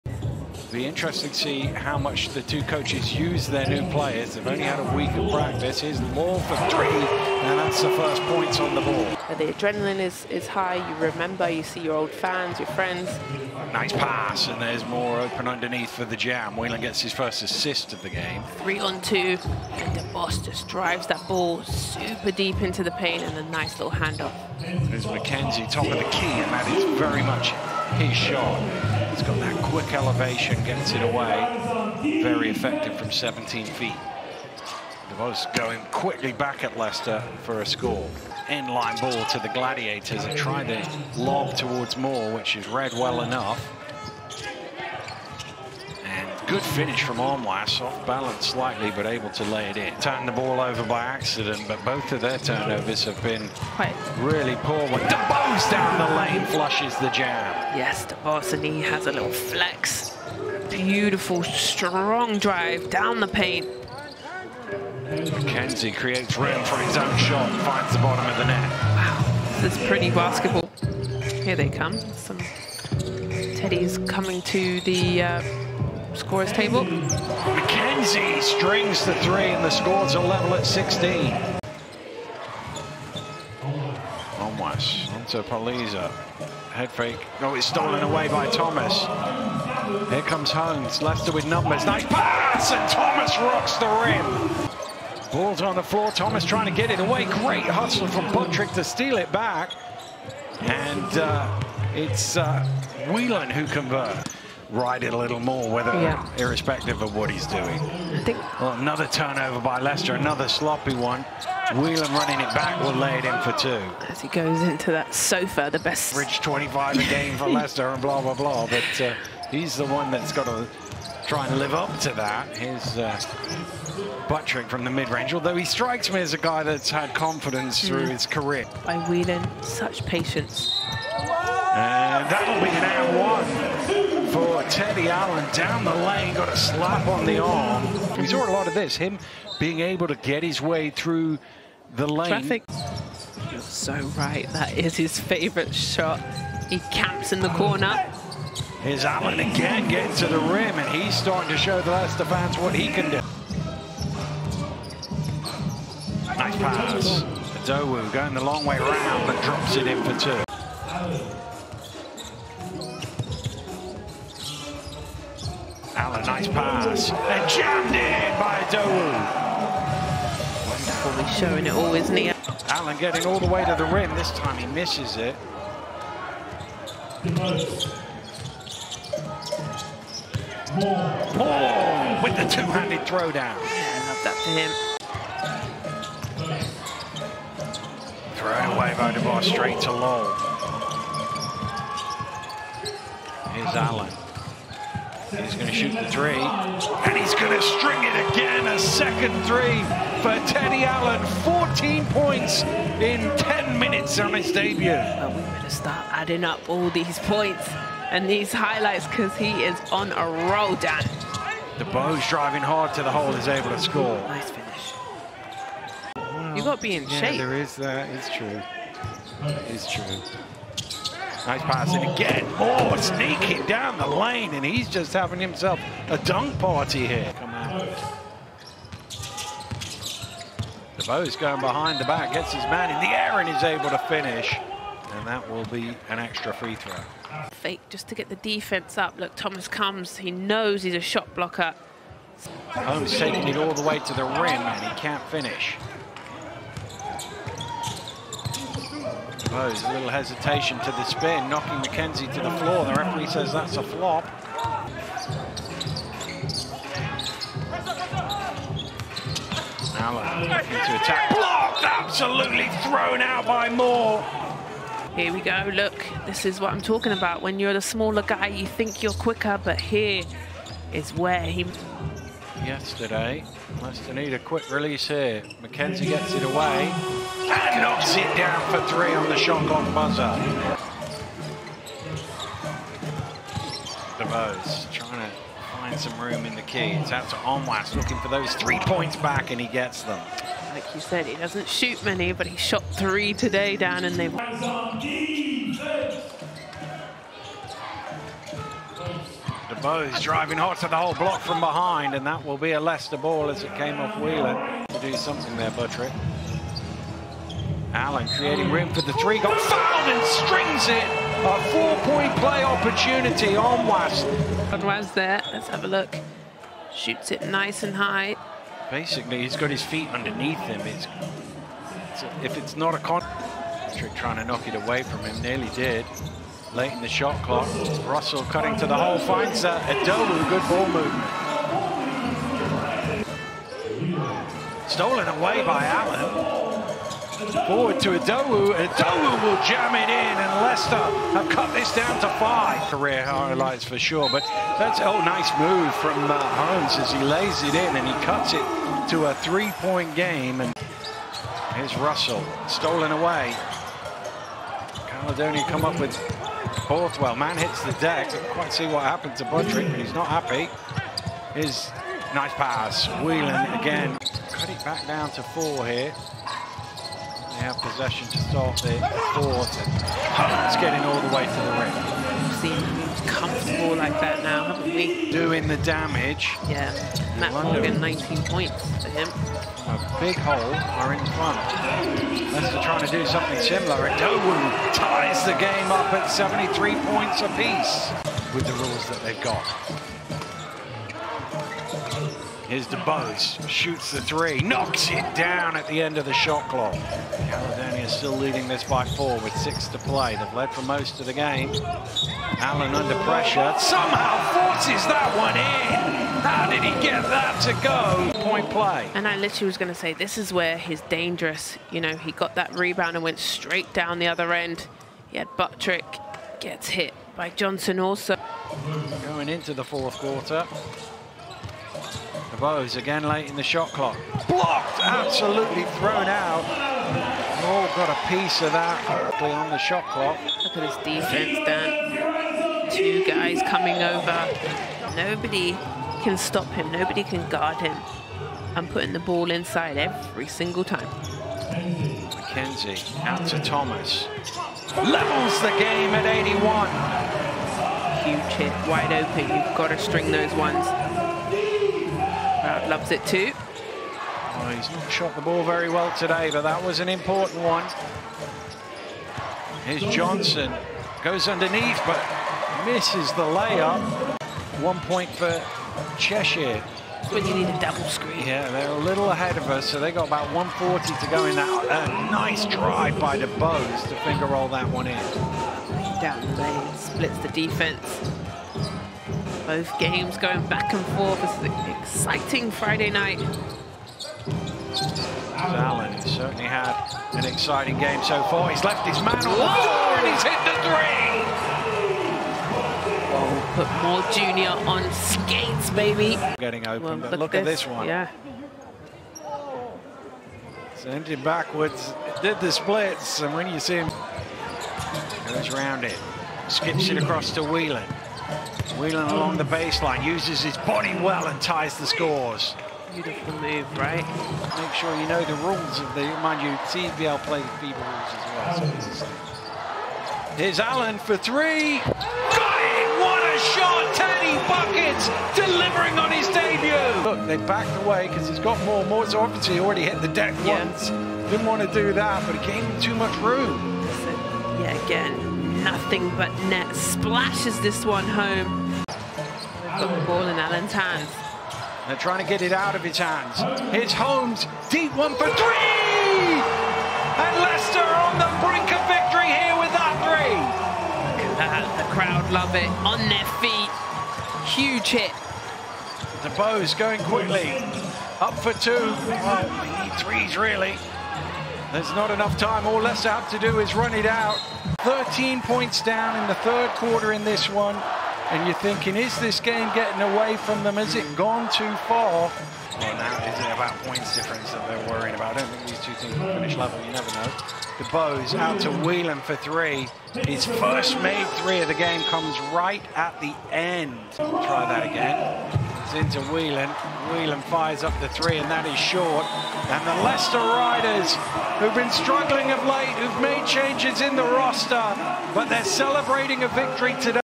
Leicester Riders vs. Caledonia Gladiators - Game Highlights
Watch the Game Highlights from Leicester Riders vs. Caledonia Gladiators, 12/15/2023

00:00:00 - 1st Quarter
00:01:56 - 2nd Quarter
00:03:47 - 3rd Quarter
00:06:04 - 4th Quarter